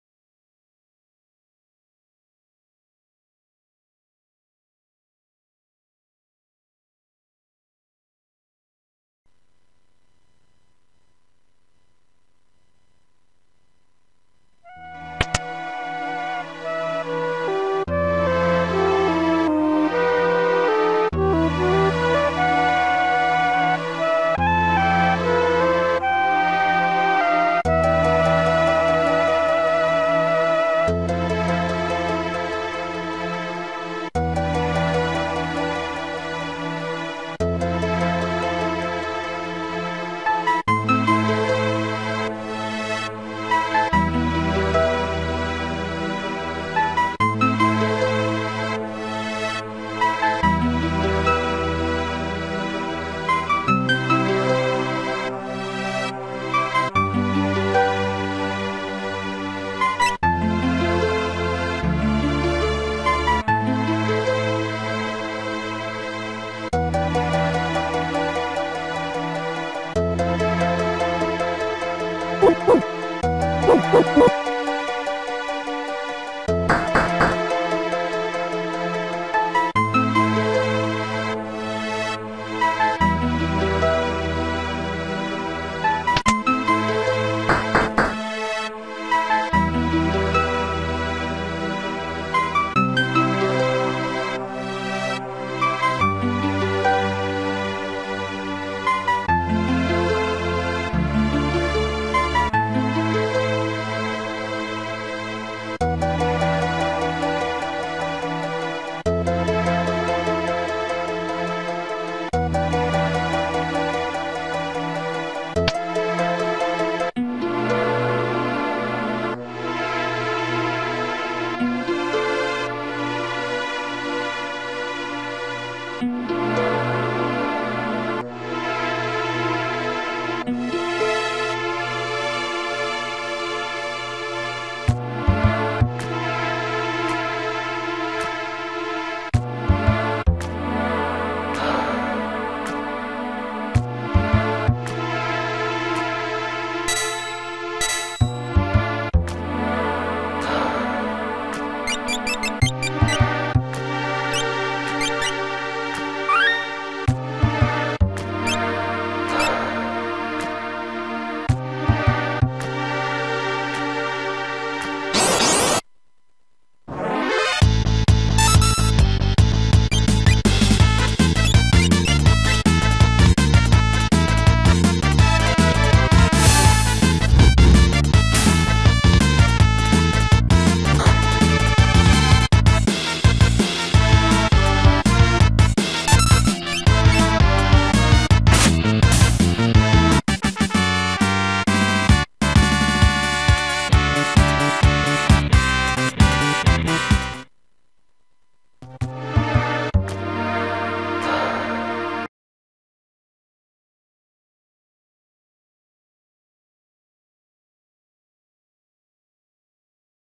SNES Emulator SNES9X Euphoria R2.1
testing the newest build of SNES9X Euphoria / SNES Emulator for PSP. Download link above.